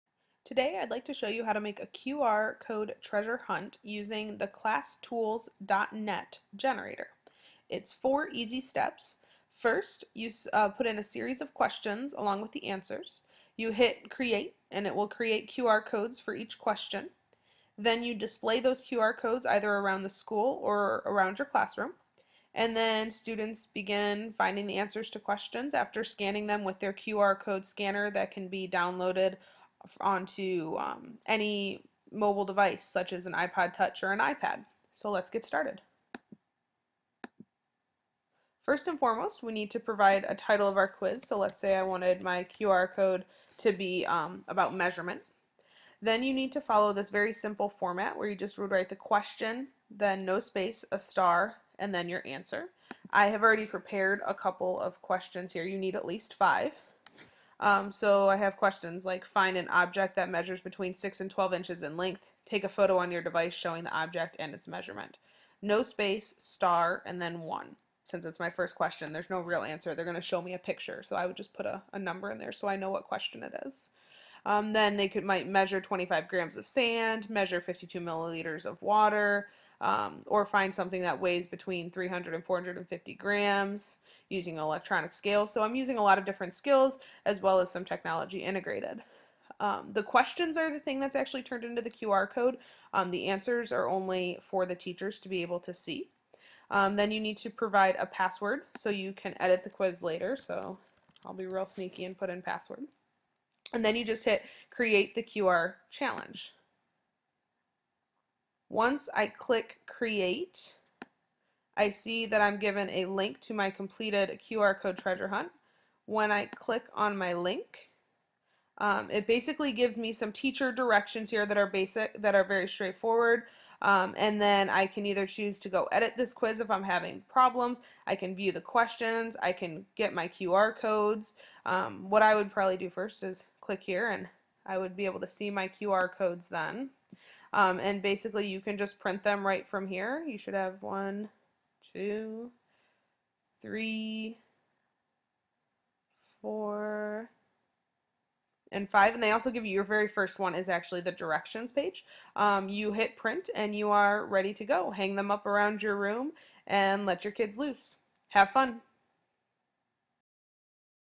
QR Code Treasure Hunt
How to video describing how to make a QR code treasure hunt.